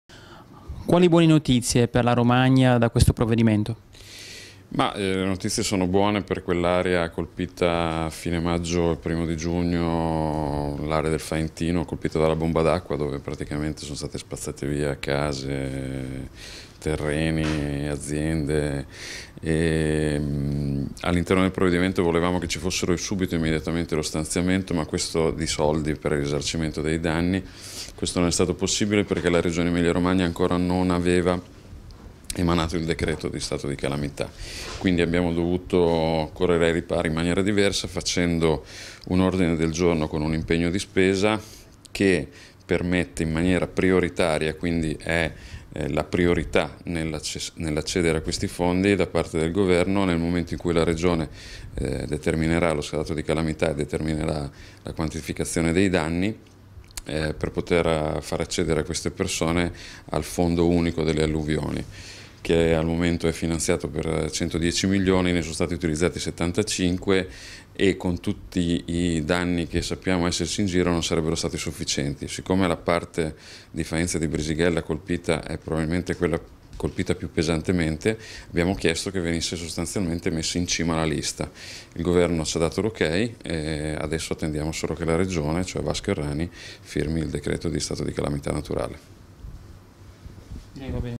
BOMBA D'ACQUA NEL FAENTINO, PINI (LN) OTTIENE OK DAL GOVERNO: "ORA SUBITO I RIMBORSI
ROMA, 12 GIUGNO - Il deputato leghista Gianluca Pini ottiene dal governo l'ok allo "stanziamento prioritario" di fondi a favore dei territori del faentino colpiti dalla bomba d'acqua del 31 maggio e 1 giugno scorso.